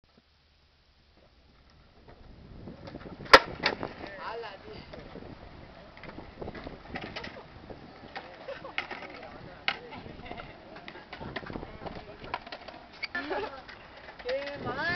viejuno moderno
en colon uno se encentra de todo, incluso a un viejo de 70 años patinando como quien no quiere la cosa